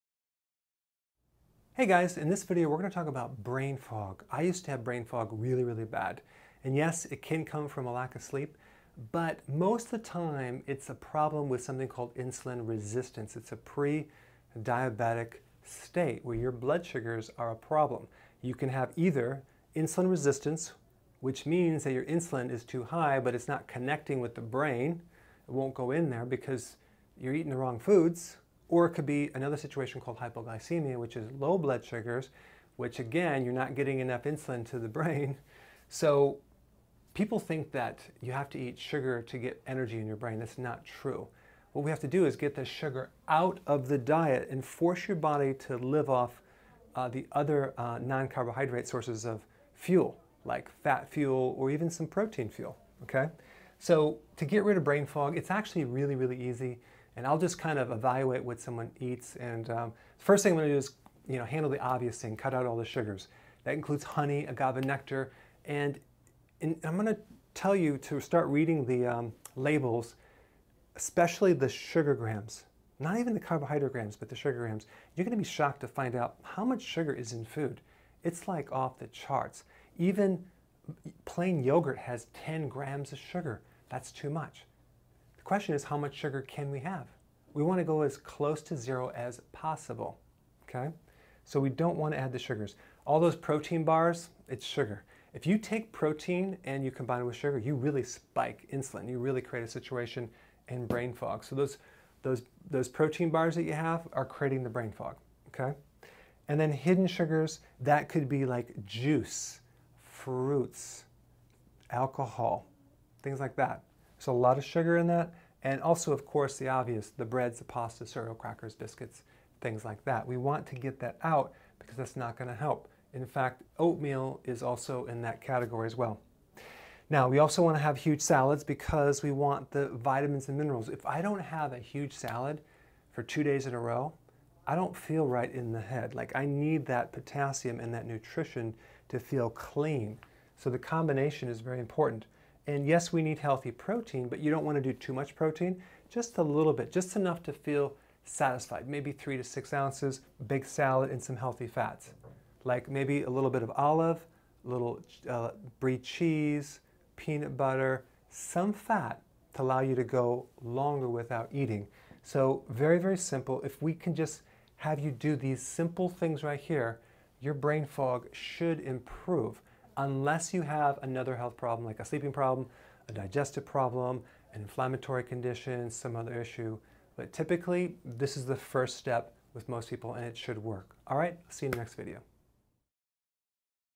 How to Get Rid of Brain Fog – Best Nutrition Tip by Dr.Berg
Dr. Berg talks about brain fog and tips on how to fix it.